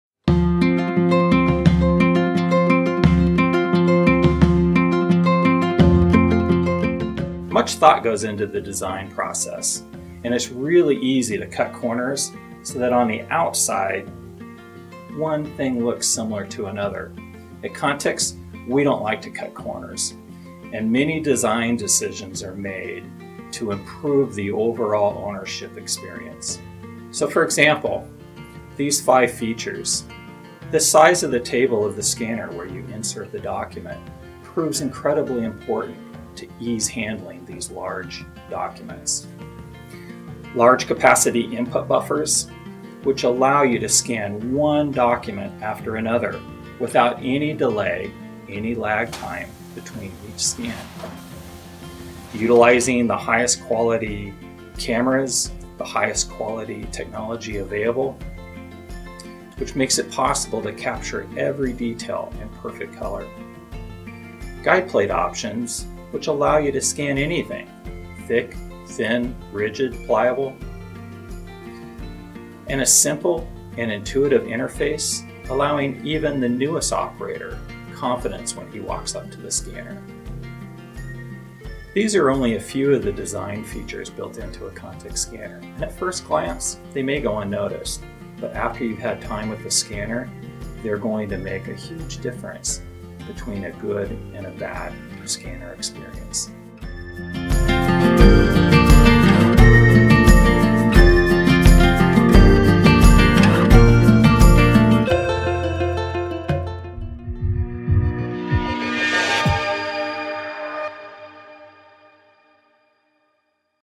Why it matters: Hardware design – Contex large format scanning
Many design features built into a Contex scanner may not be noticeable at first glance. However, once you put the scanner to use, together they make all the difference between a good and a bad scanning experience.

This video explains five hardware design features that we have made to improve your overall ownership experience.

Contex A/S is the leading manufacturer of large format scanners and large format scanner software for both color and monochrome scanning solutions.